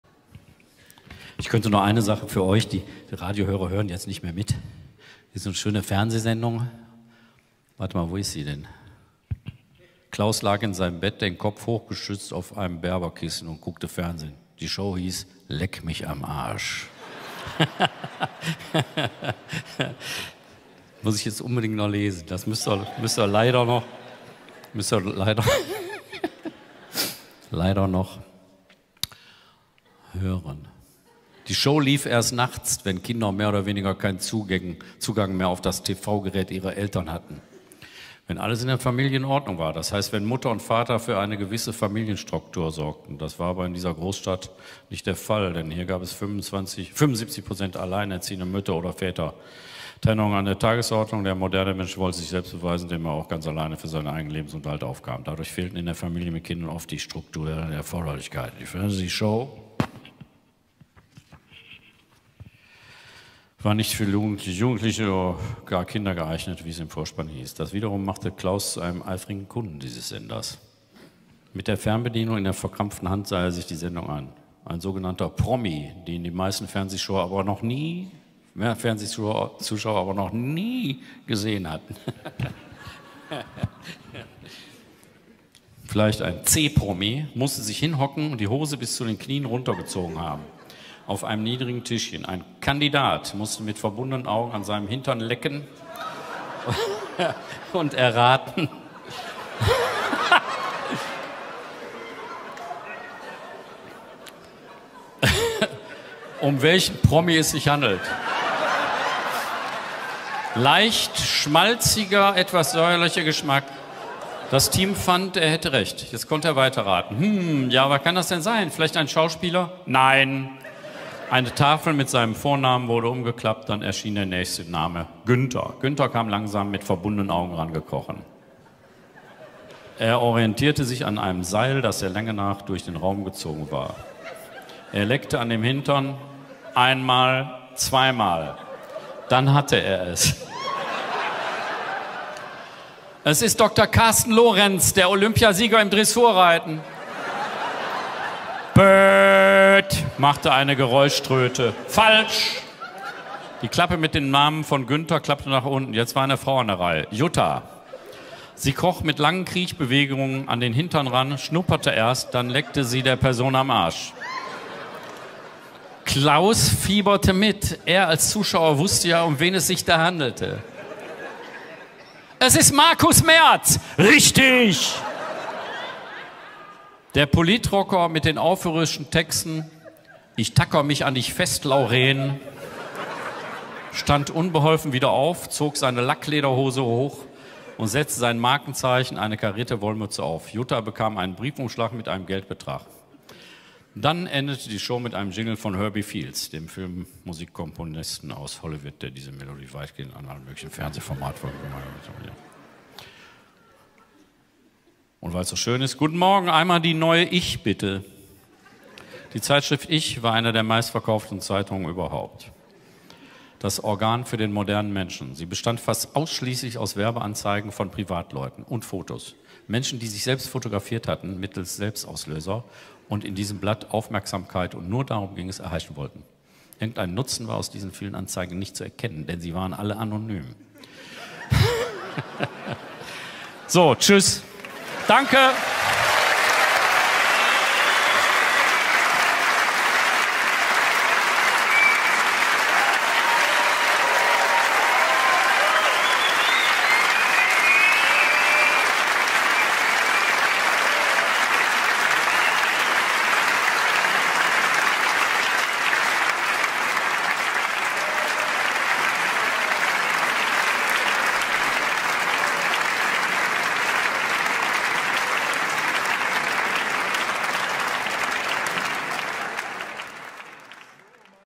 Helge Schneider Buchlesung "Stepptanz" TV Show
Lesung im RBB Sendesaal Berin am 14.10.23